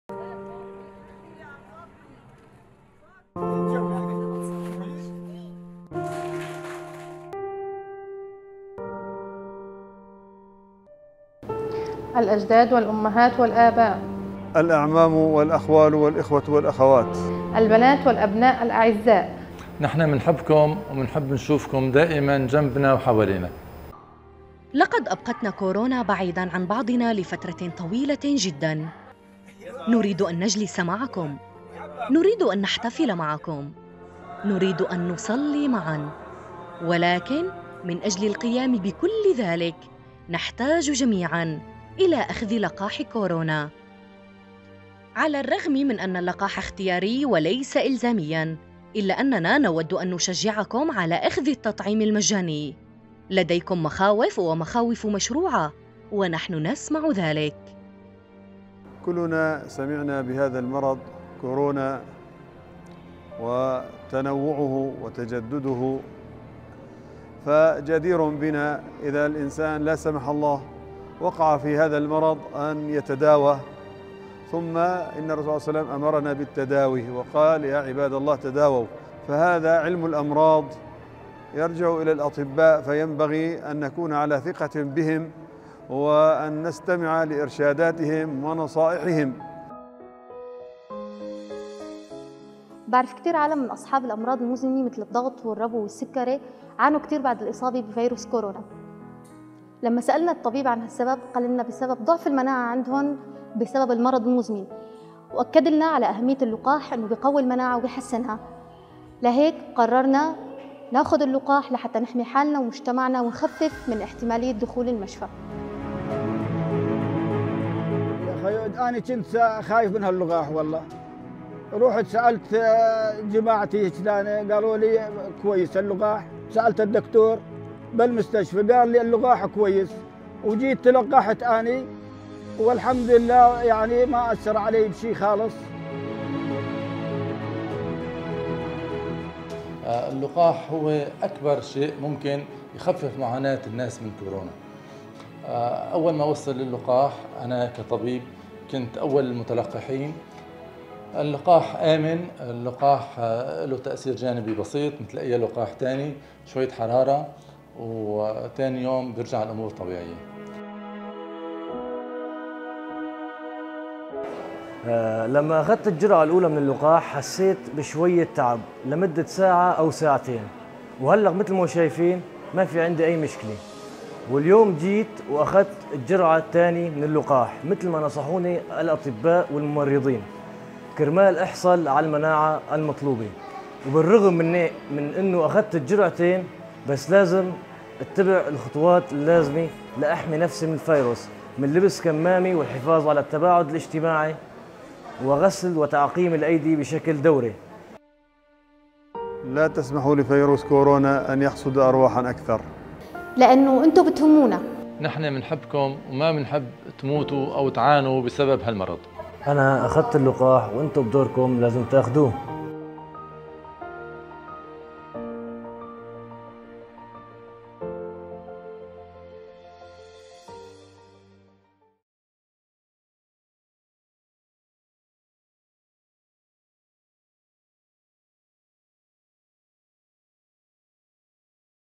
COVID-19 Vaccine Community Mobilization in northwest Syria
It's been more than a year since COVID-19 upended our social life.
Our future is in our hands. By getting vaccinated, we have the power to end the pandemic faster.

No one is safe until everyone is safe.

IOM Turkey has launched an awareness-raising video on COVID-19 vaccination in northwest Syria to support the Health Cluster's community mobilization efforts to dispel misconceptions about vaccines and encourage people to get vaccinated.

The video aims to show that the vaccine is safe and build trust in the communities using testimonies of key community members vaccinated against COVID-19.

The Health cluster is based in Turkey and  consists of UN agencies, INGOs, NGOs thematic task force and working groups to address public health issues in Syria.